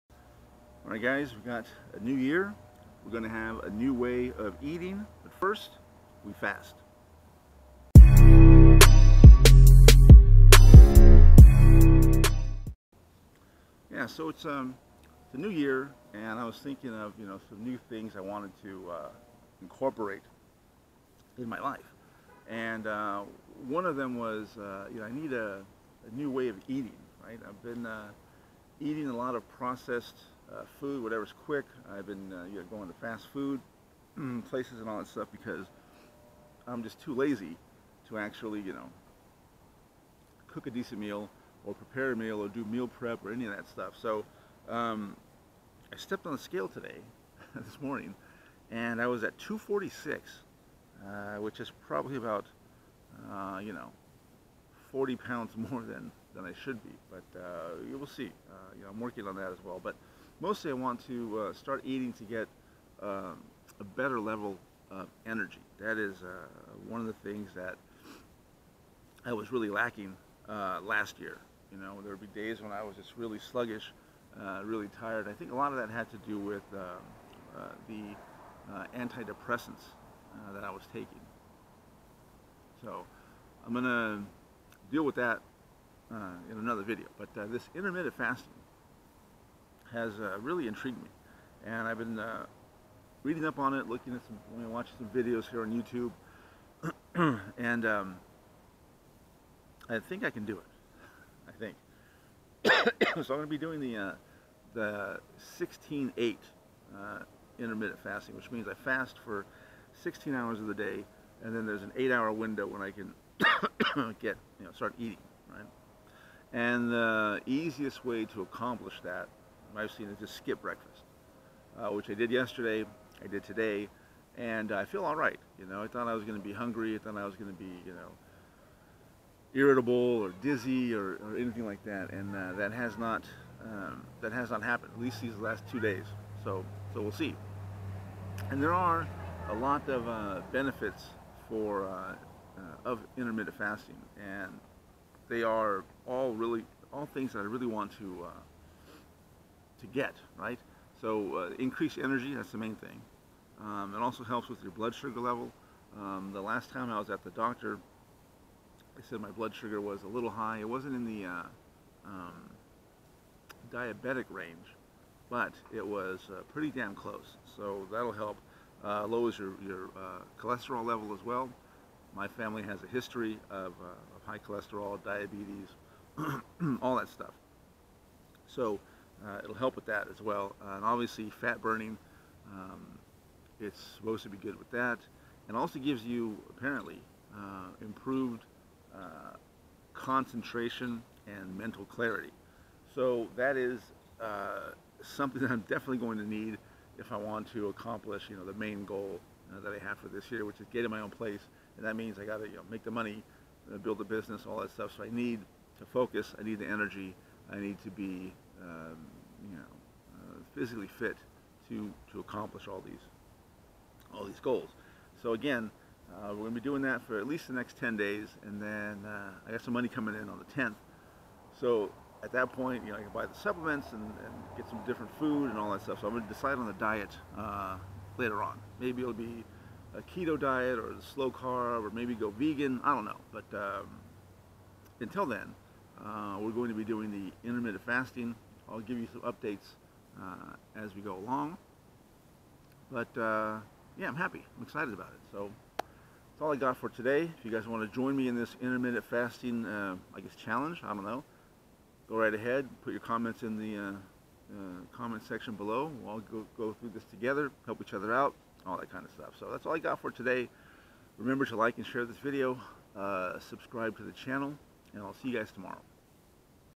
Vlog 2 JAN 19 - Where I Try Some Intermittent Fasting
I am going to be experimenting with intermittent fasting over the next 10 days to see how it will affect my overall health and energy.

I think this will be a very interesting experience.